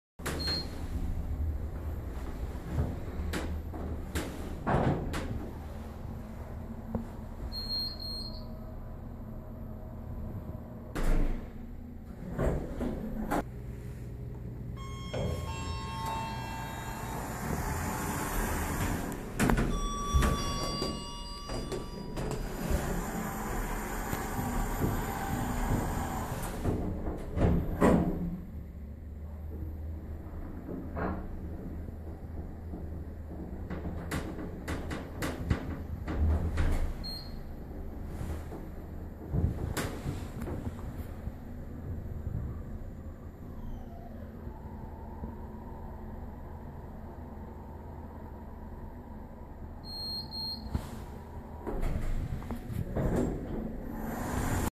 Kone Hydraulic Elevators at SMC
1 of 2 early 00's Kone hydraulic elevators at the Southdale Medical Center in Edina, MN.  This was one of the last years that Kone made hydraulic elevators before moving entirely to the Ecodisc.  Short video.